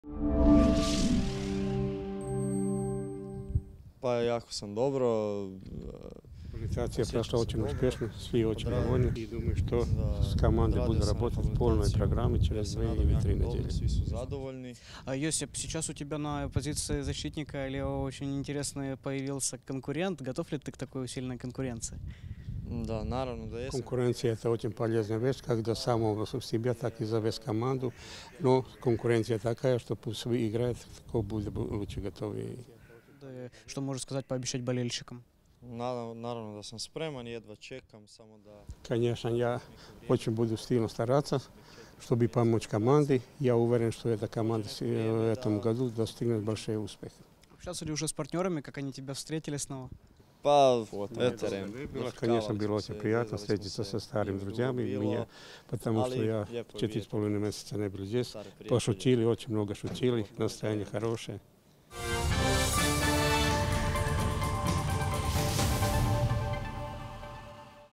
ПІВАРИЧ рветься на поле!
Лівий захисник київського "Динамо" Йосип ПІВАРИЧ дуже близький до повернення на поле. В інтерв'ю Dynamo Web TV хорват розповів про ступінь своєї готовності, конкуренцію на позиції та про те, як партнери зустріли його у команді!